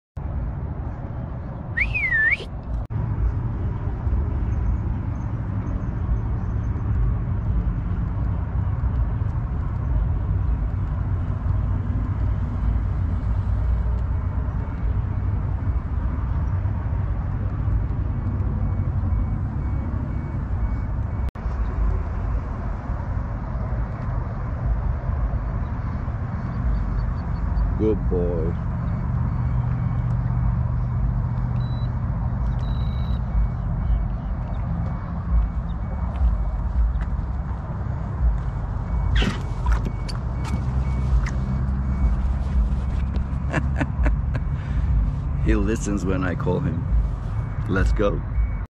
Old Town Sportsman Autopilot 120 Kayak with Minn Kota Motor! Remote Control Trick! 😎
Old Town Sportsman Autopilot 120 fishing kayak remote control trick! You can control your kayak remotely with the Minn Kota motor GPS and Bluetooth technology from a distance! One of the incredible features of this amazing game changer fishing kayak!